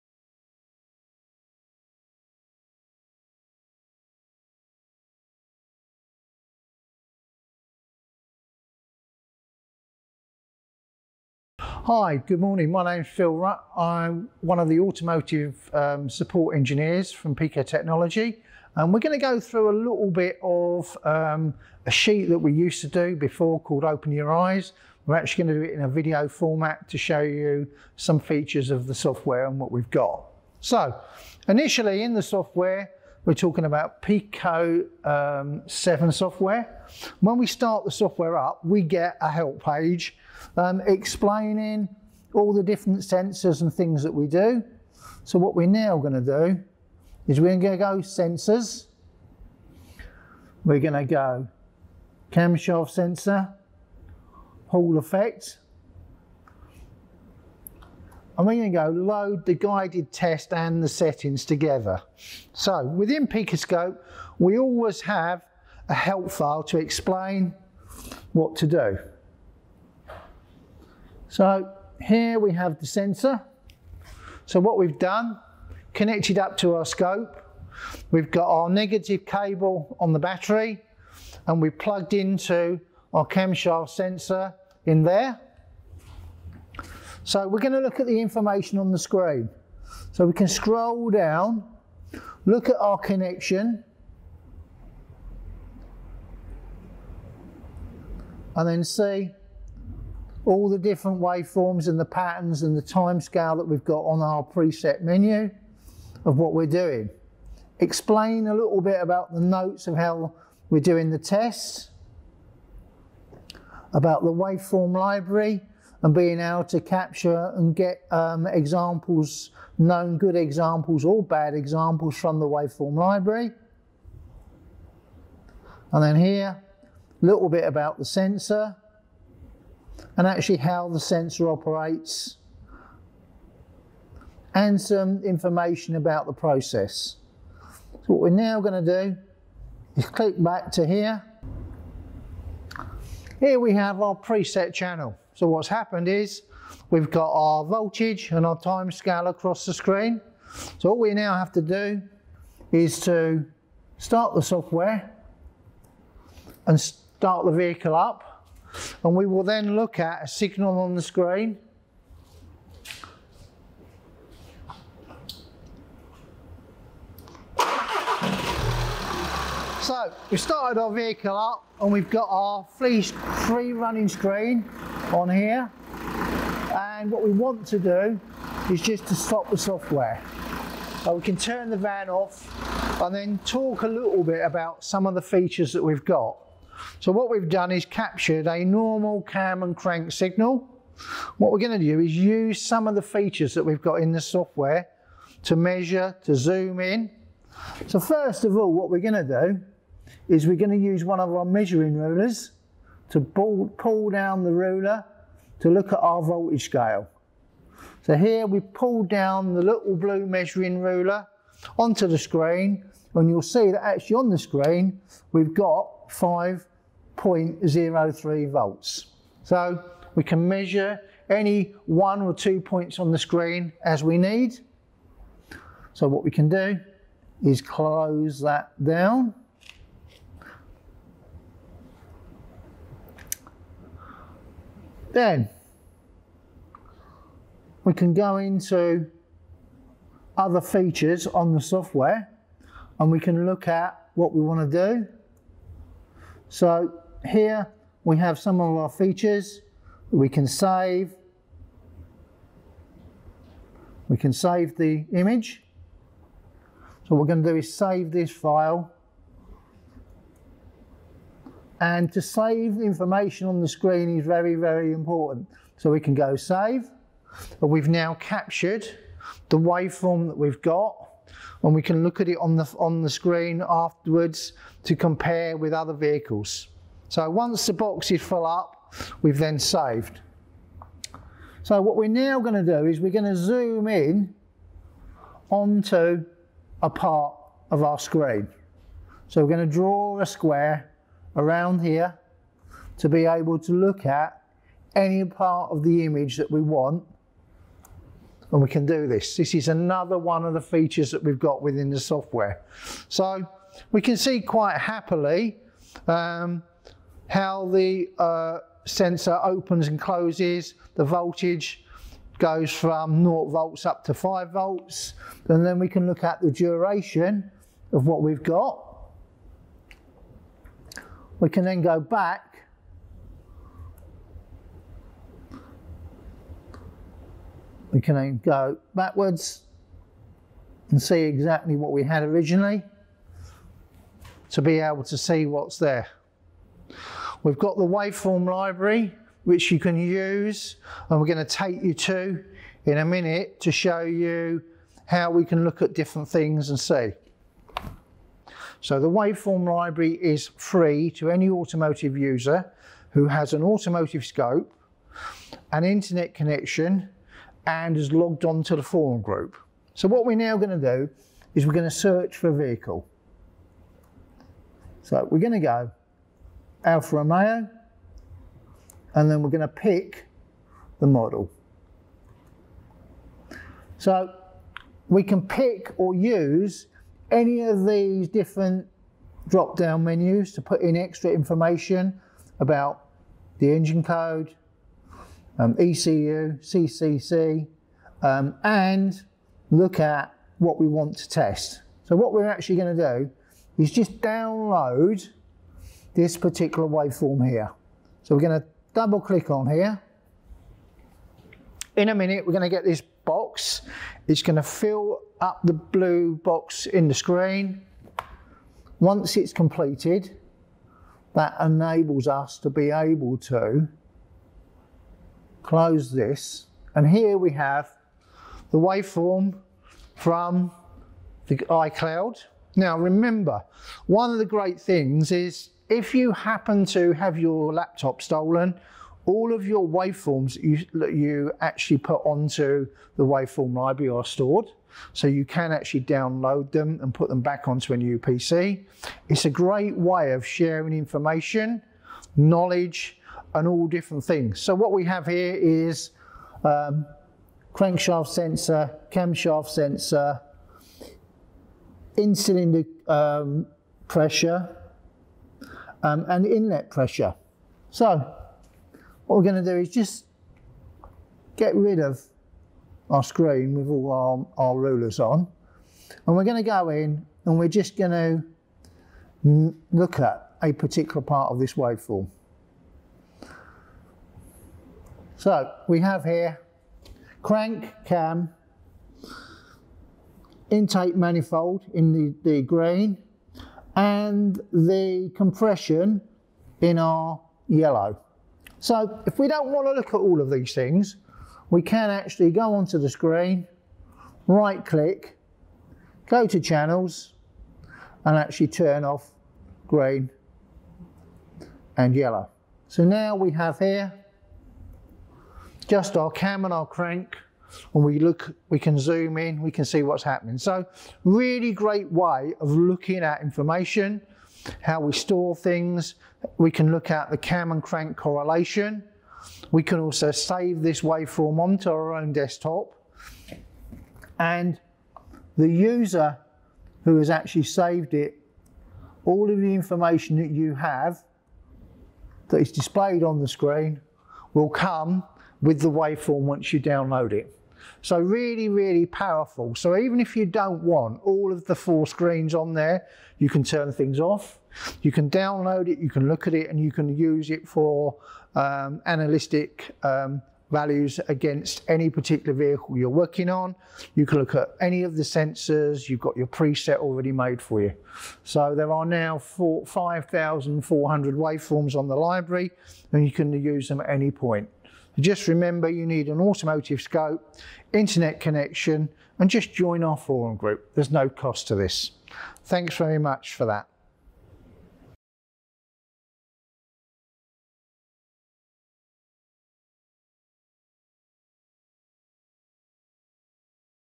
PicoScope Automotive: Open Your Eyes
In this video Phil Rutt, an Automotive Support Engineer at Pico Technology presents a video giving an overview of PicoScope Automotive in action.

Phil starts wth capturing a Cam & Crank signal from the Pico van, then uses the PicoScope 7 Automotive tools to analyse the collected waveform. While saving, zooming, measuring and sharing the waveform, Phil shows how to best use PicoScope to maximise your understanding of the captured data.